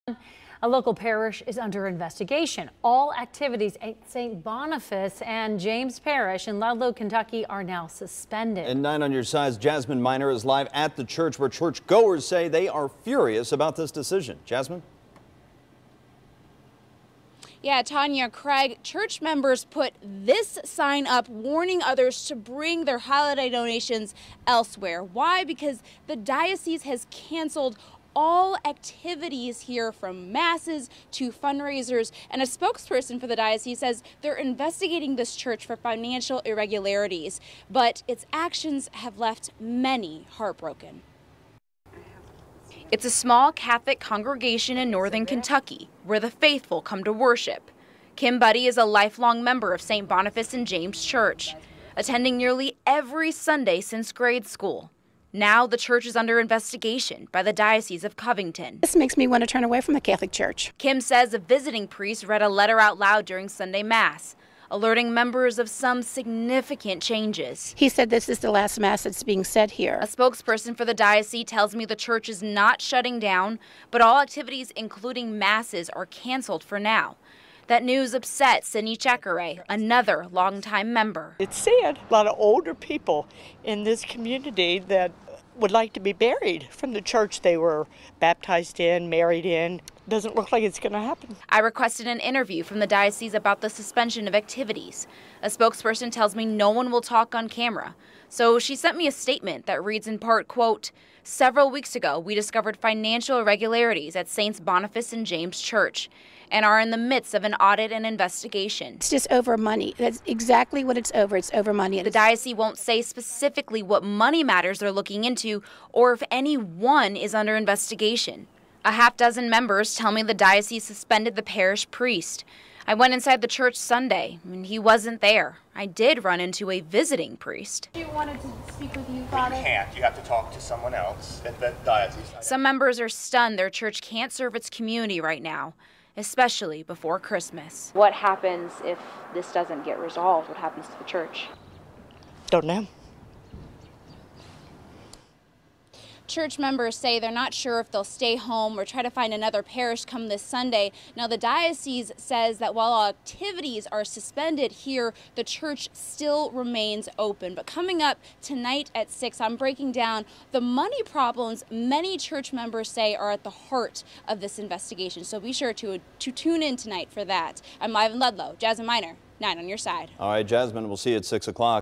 Ludlow church under investigation
Saints Boniface and James Roman Catholic Parish is not closing, according to a Diocese of Covington spokesperson; however, all activities at the parish are suspended due to "financial irregularities" discovered several weeks ago.